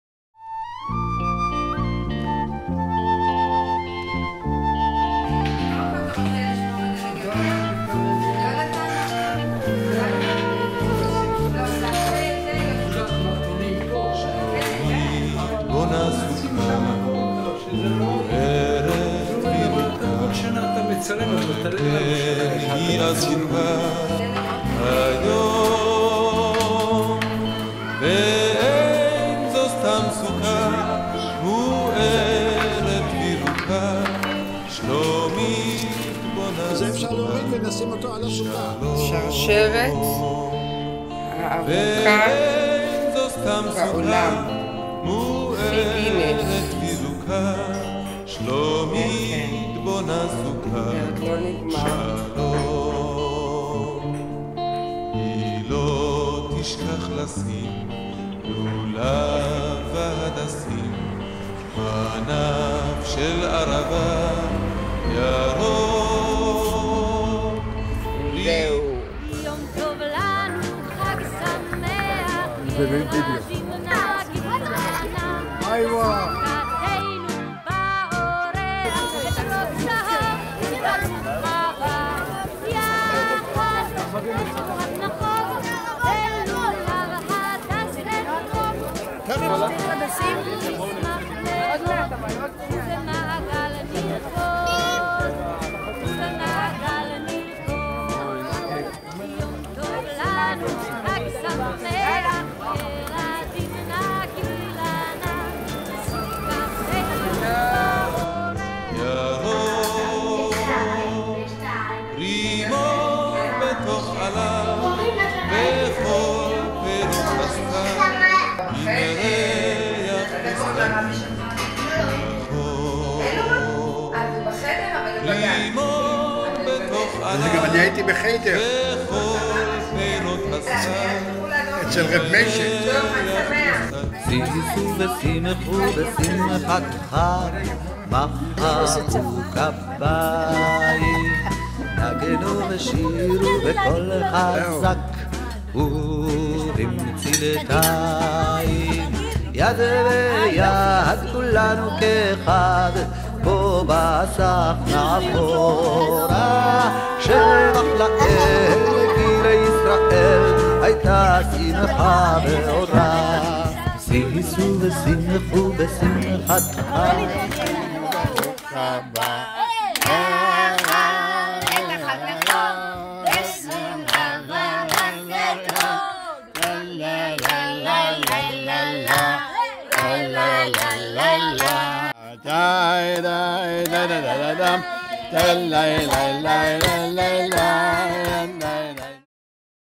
סוכות תש"פ בנייה, קשוט, 4 מינים בבית וגן י"ד תשרי 13 באוקטובר 2019
סוכות תש"פ בבית הרמתי. בנייה, קשוט, 4 מינים בבית וגן י"ד תשרי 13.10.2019
צילום ועריכה: ראובן הרמתי